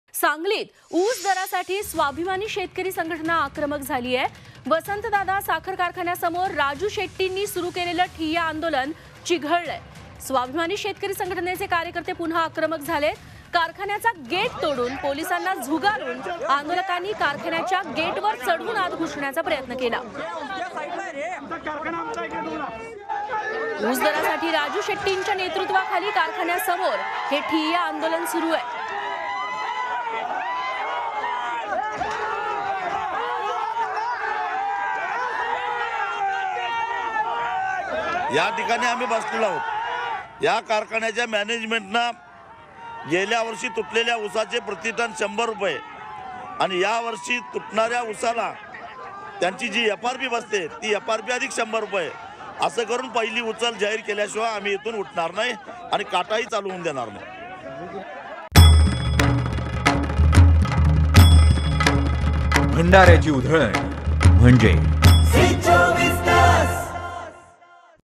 Raju Shetty | सांगलीतील ऊस दरासाठी स्वाभिमानी शेतकरी संघटनेचं आंदोलन चिघळलं, गेट तोडण्याचा प्रयत्न
In Sangli, self-respecting farmers' organization has become aggressive for the price of sugarcane. Activists of Swabhimani Shetkar Sangathan have become aggressive again.. After breaking the gate of the factory, defying the police, the protestors climbed the gate of the factory and tried to enter.

सांगलीत ऊस दरासाठी स्वाभीमानी शेतकरी संघटना आक्रमक झालीये.. वसंतदादा साखर कारखान्यासमोर राजू शेट्टींनी सुरू केलेलं ठिय्या आंदोलन चिघळलंय. स्वाभिमानी शेतकरी संघटनेचे कार्यकर्ते पुन्हा आक्रमक झालेत.. कारखान्याचा गेट तोडून पोलिसांना झुगारुन आंदोलकांनी कारखान्याच्या गेटवर चढून आत घुसण्याचा प्रयत्न केला.. उस दरासाठी राजू शेट्टीच्या नेतृत्वाखाली कारखान्यासमोर ठिय्या आंदोलन सुरु आहे..